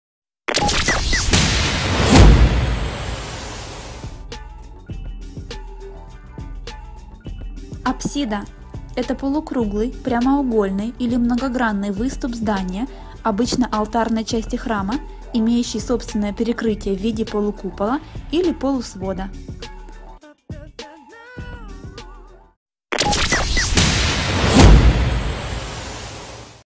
Апсида. Толковый Видеословарь русского языка
Что такое апсида. Дефиниция слова апсида. Апсида в Толковом Видеословаре русского языка.